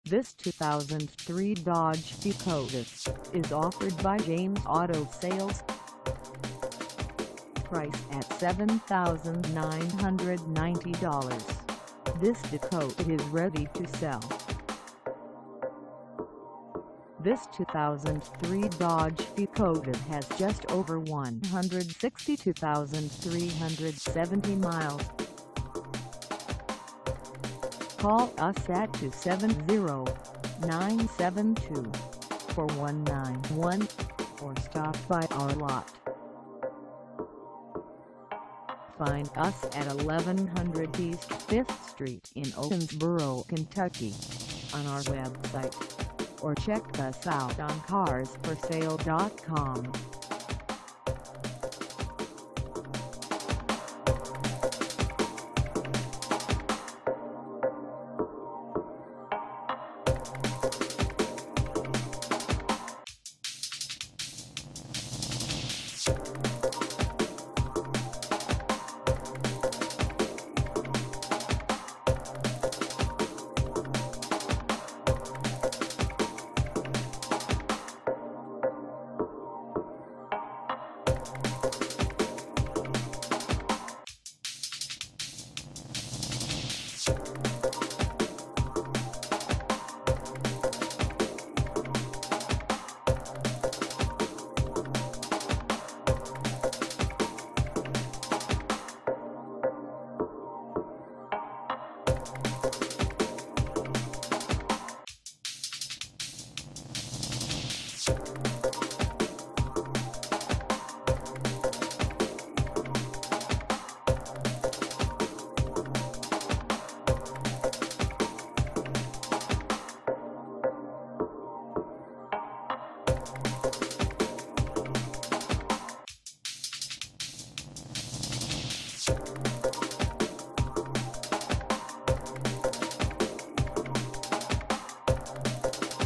2003 Dodge Dakota SLT Quad Cab 4WD - for sale in Owensboro,
James Auto Sales

Come test drive this 2003 Dodge Dakota SLT Quad Cab 4WD for sale in Owensboro, KY.

Back in the day, there was only one game in town if you wanted a mid-sized truck - Meet the 2003 Dodge Dakota 4X4 Club Cab Sport that comes with a 3.9-liter V6 that puts out plenty of power and gives you the sure feel of the road with a five-speed manual transmission. The Dodge Dakota was the first pickup truck to feature rack and pinion when it was introduced in 1987 just in case you wanted a quick fact. The Dakota trucks just like this one are popular as both a truck and a second family car. As you can see, it combines the comfort and easy drivability of a compact truck with the power and hauling capacity of a full-size. The front of the cab is spacious, with a well-laid-out dash featuring large controls. Drivers love the power of the 3.9-liter V6 engine and the control of the gears. This may be a base truck to some but the reality is that is a well rounded machine suited for any driver.